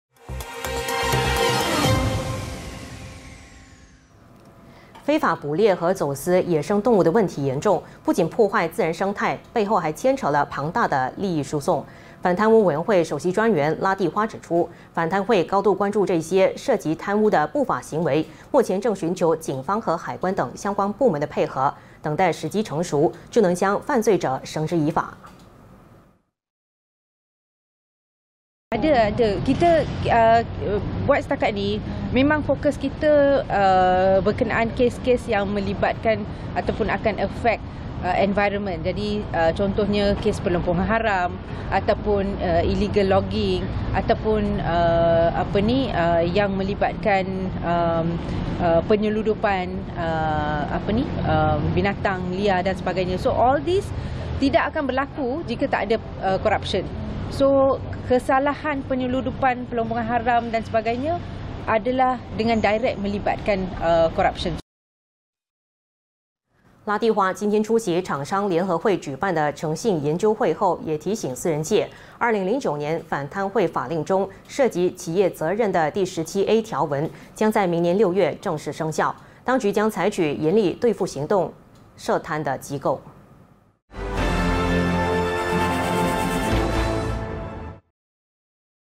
20191204 非法猎捕涉贪猖獗 反贪会寻求警方合作打击
非法猎捕和走私野生动物的问题严重，不仅破坏自然生态，背后还牵扯了庞大的利益输送。反贪污委员会首席专员拉蒂花指出，反贪会高度关注这些涉及贪污的不法行为，目前正寻求警方和海关等相关部门配合等待时机成熟，就能将犯罪者绳之于法。